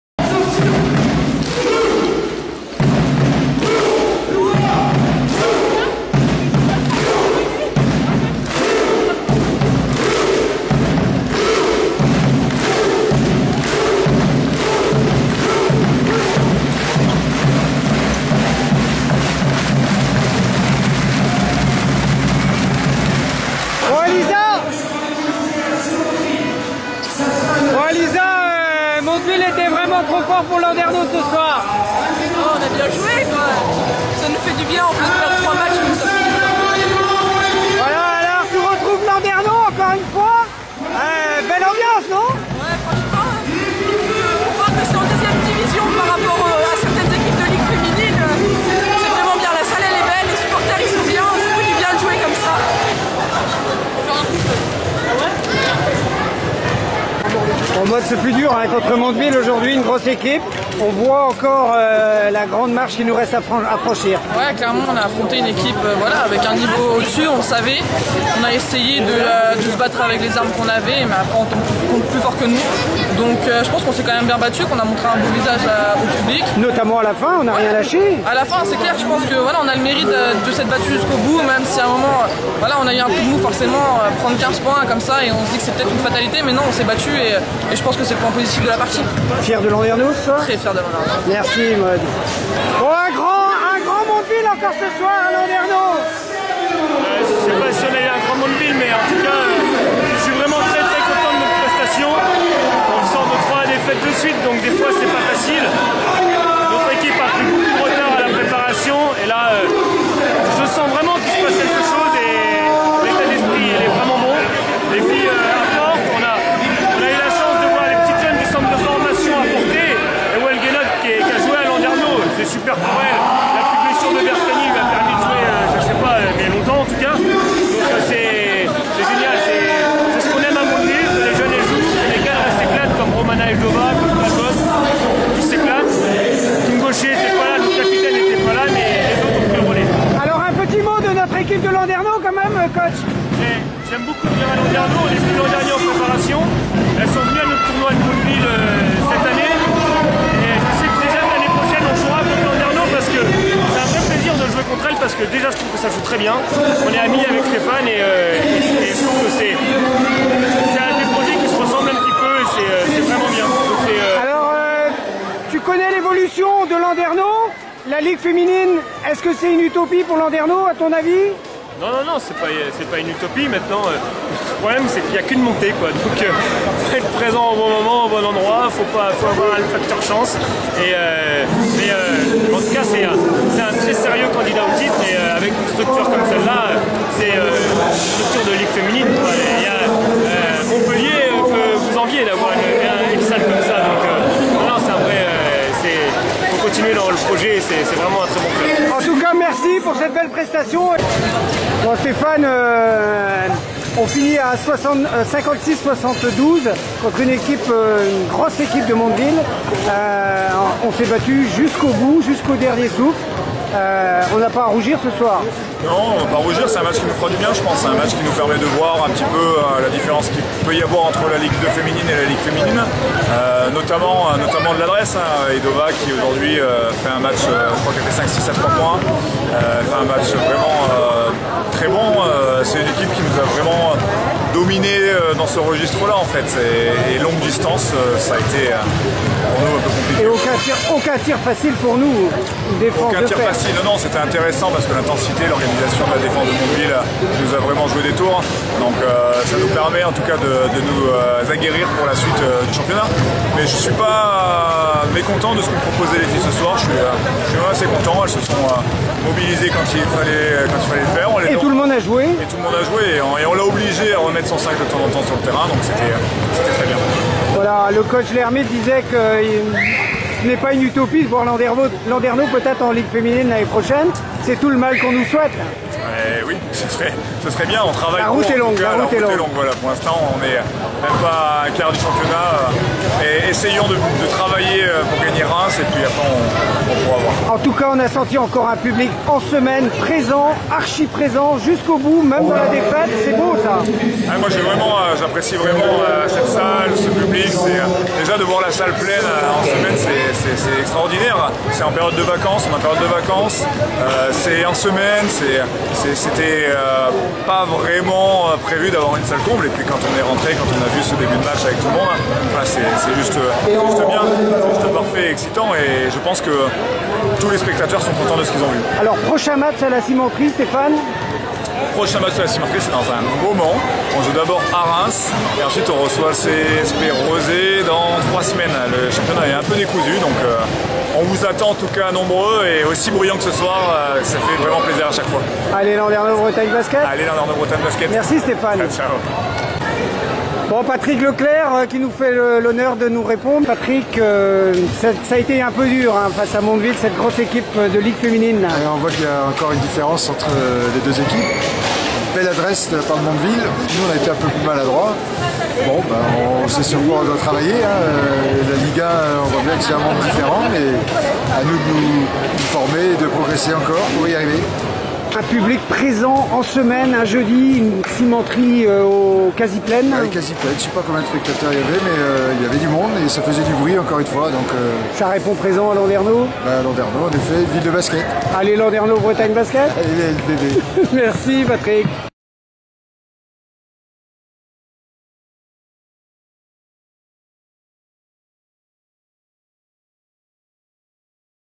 CDF Jeudi 26 Octobre 2017 LBB vs USO Mondeville : Après-Match
CDF Jeudi 26 Octobre 2017 LBB (LF2) vs USO Mondeville (LFB) : Après-Match
1/16 ème de Finale
Victoire de USO Mondeville 72 à 56
Salle presque comble